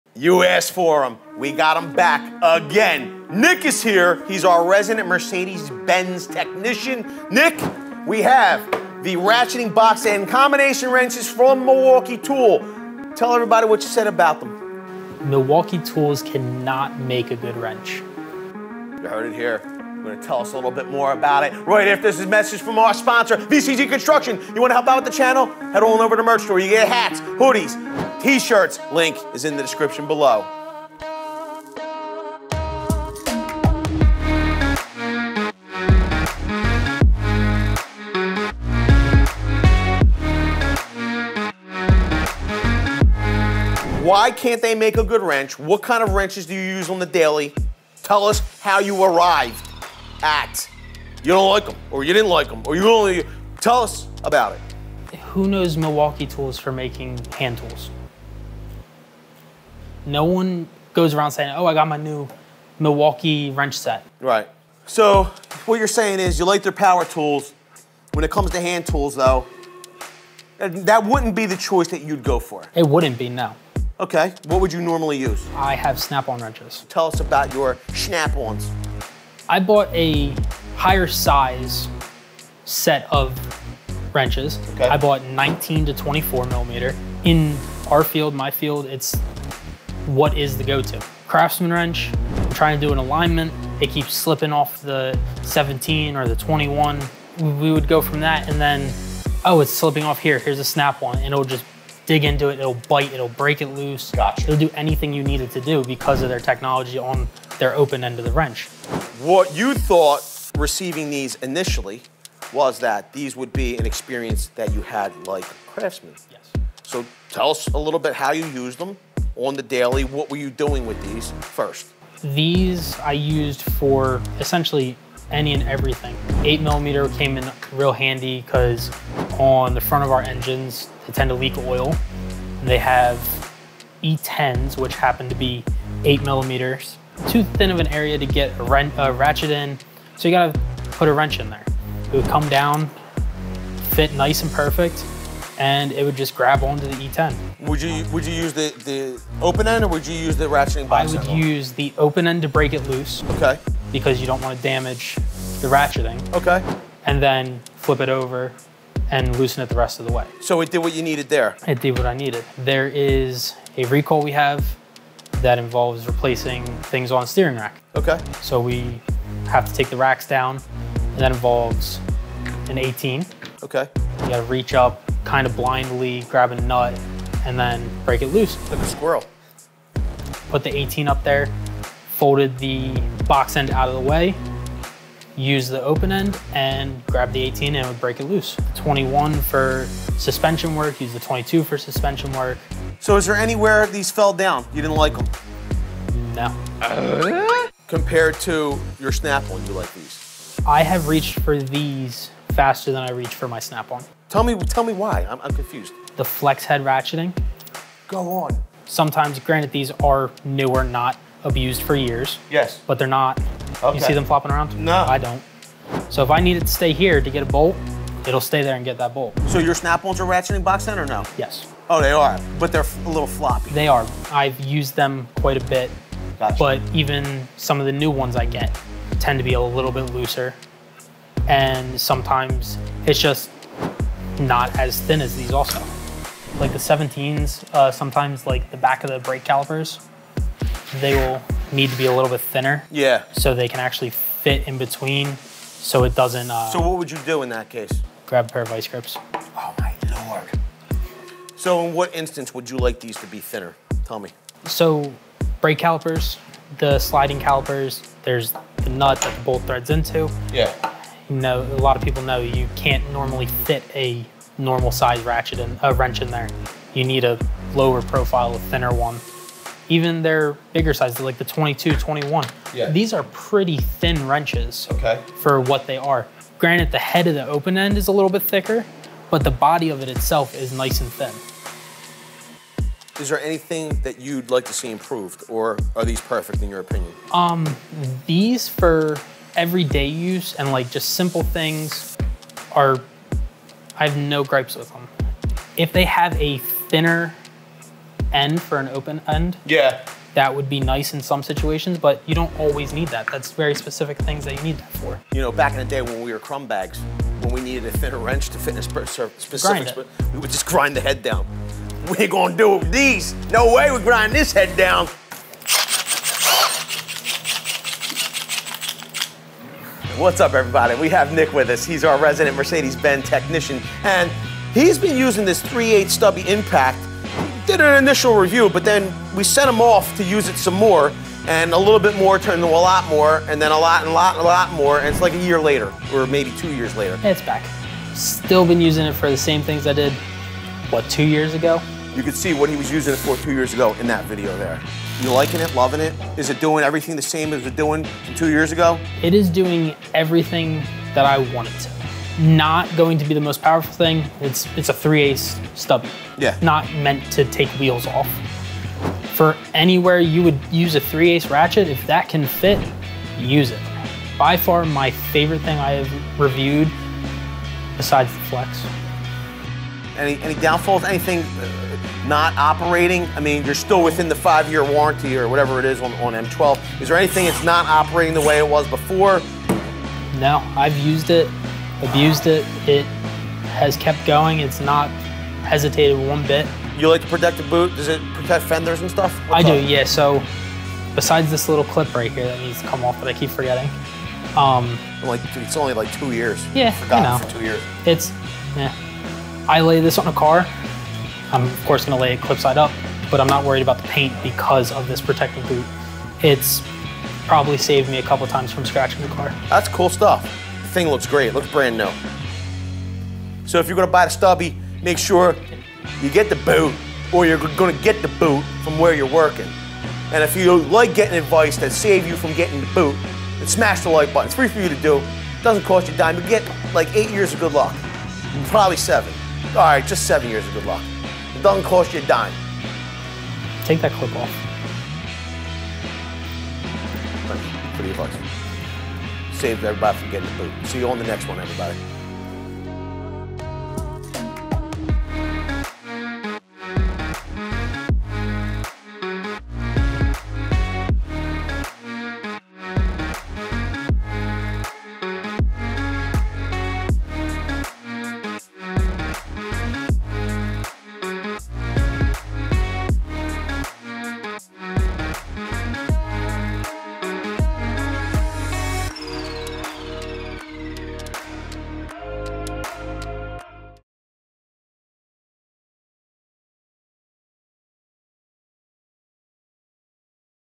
Milwaukee Compared To Snap On Tools (NOT WHAT YOU THINK)
Milwaukee HAND TOOLS FOR MECHANICS Compared To Snap On combination wrenches with ratcheting box end Tools (NOT WHAT YOU THINK) we compare Milwaukee tools 15pc Metric Flex Head Ratcheting Combination Wrench set (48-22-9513) to the equivalent Snap-On 6 pc 12-Point Metric Flank Drive Plus 0° Offset Non-Reversing Ratcheting Combination Wrench Foam Set (17–24 mm) combination wrenches and the results are not what you think!